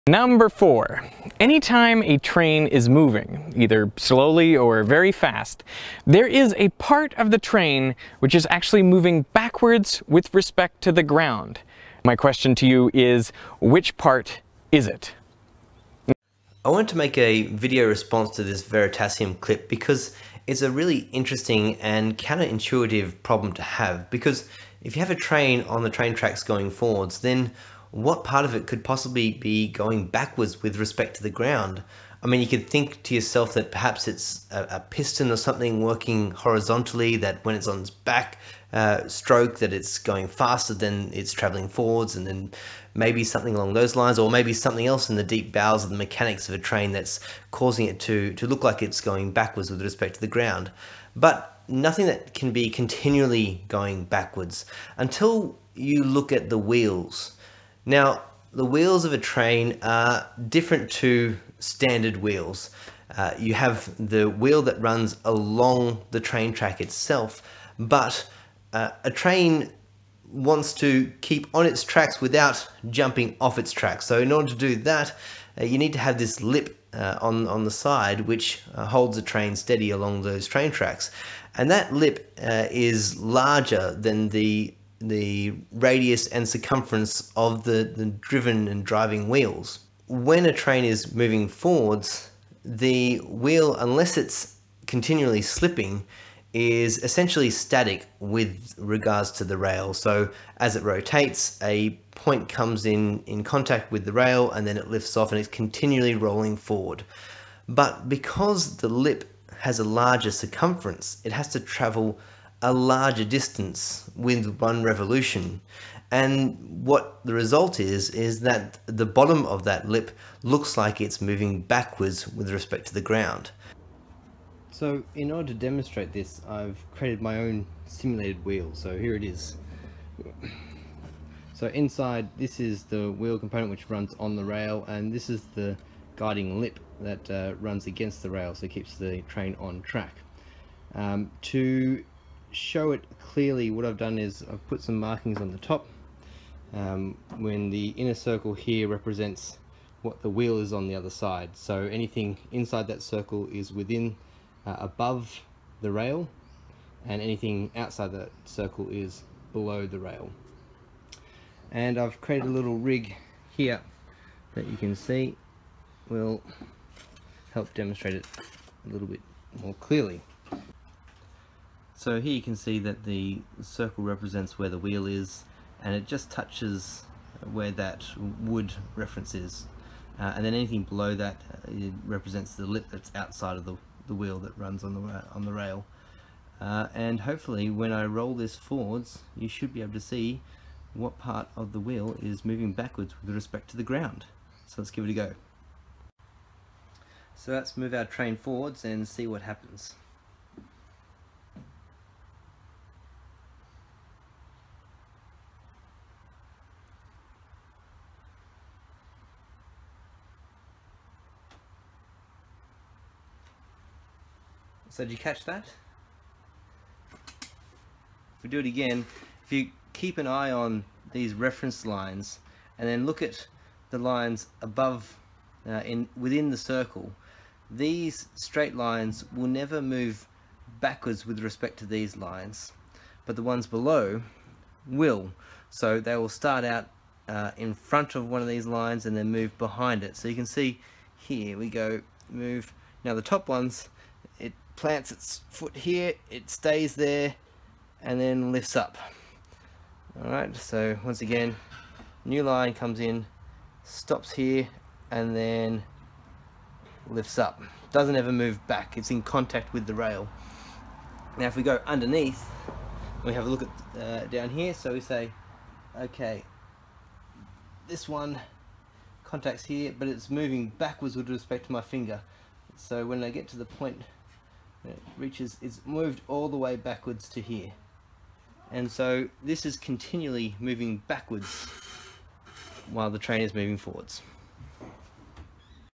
Part of a train always travels backwards! Veritasium riddle 4 answered
it shows how there is always a part of a train that is travelling backwards with respect to the ground